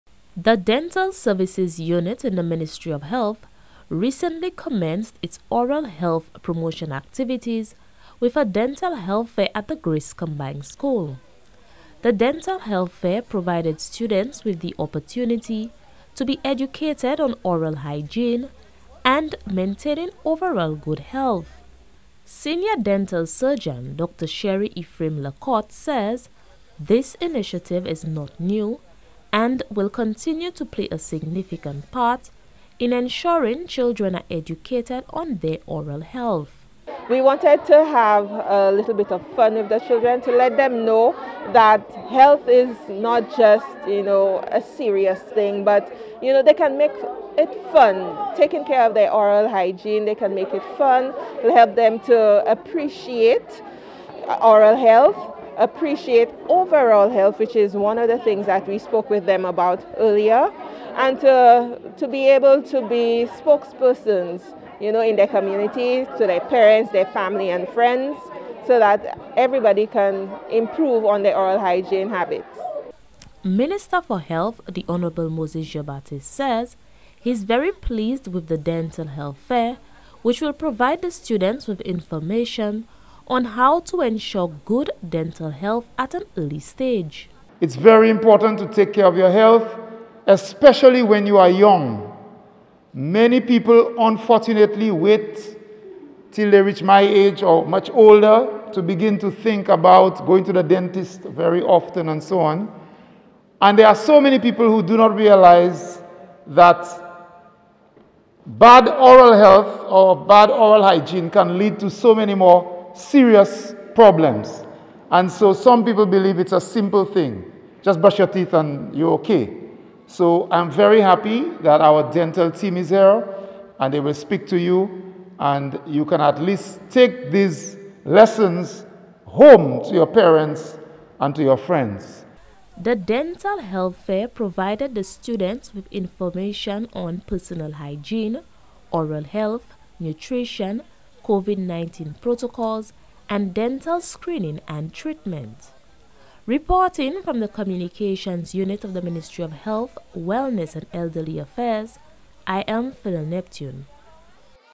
Dental Health Fair at the Grace Combined School - Ministry of Health Report
Students were granted the opportunity to receive valuable information on oral health.

The Dental Services Unit in the Ministry of Health recently commenced its oral health promotion activities with a dental health fair at the Grace Combined School.